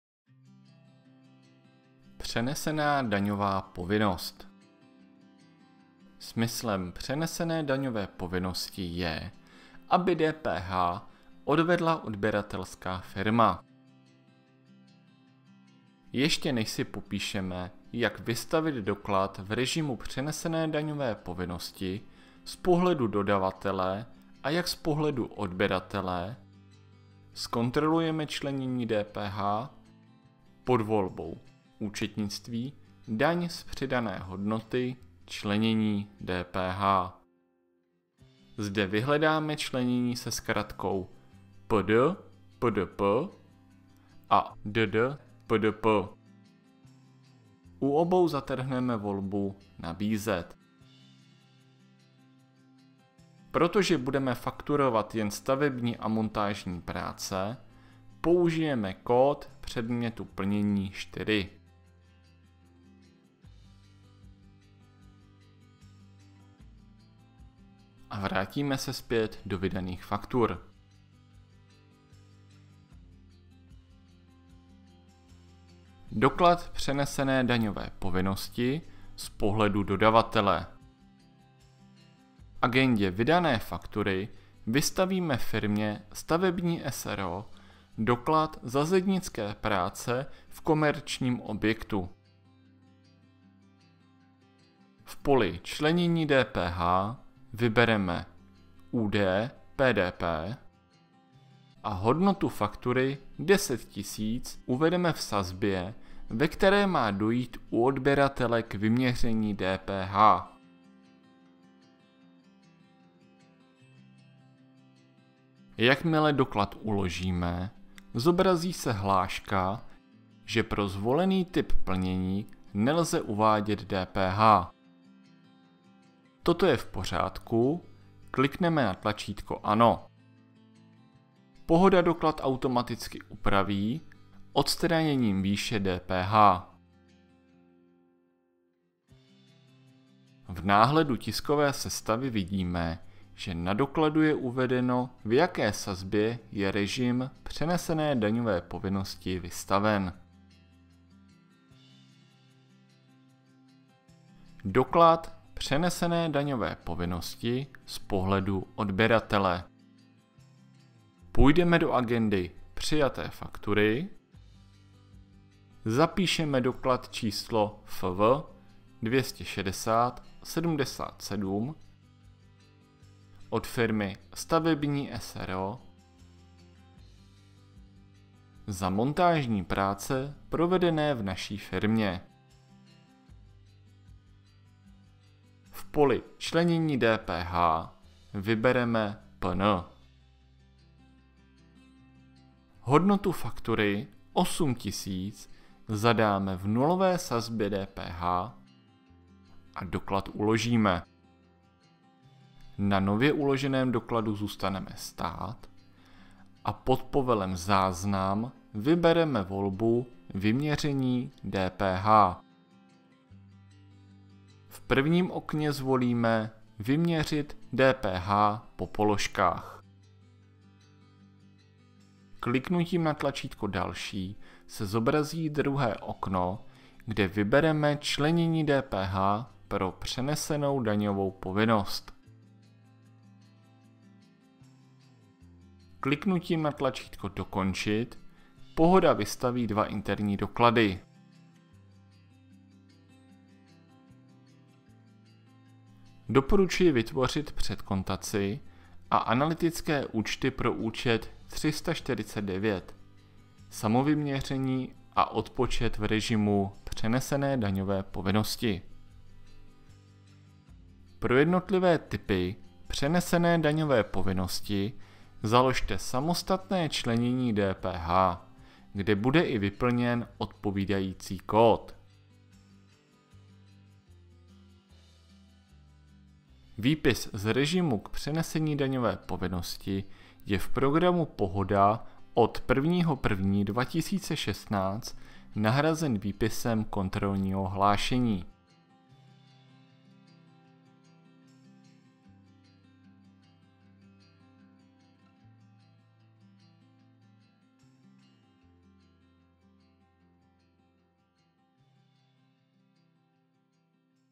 POHODA: Správné zpracování dokladů s přenesenou daňovou povinností
Naučte se efektivně pracovat s doklady, kde je uplatněna přenesená daňová povinnost, v programu POHODA. Tento tutoriál vás provede celým procesem, od evidence dokladu až po správné zaúčtování, což vám pomůže zajistit soulad s daňovými předpisy.